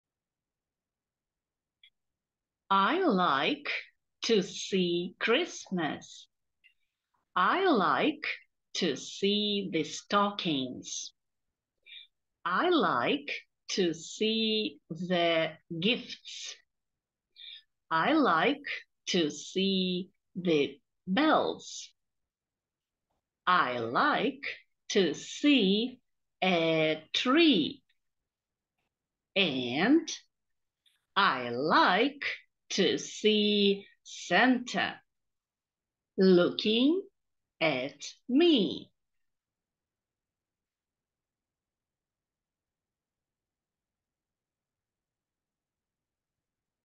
A Christmas poem! Short and easy to learn!
I like to See Christmas

I like to see the stockings.
I like to see the gifts.
I like to see the bells.
I like to see a tree.
And I like to see Santa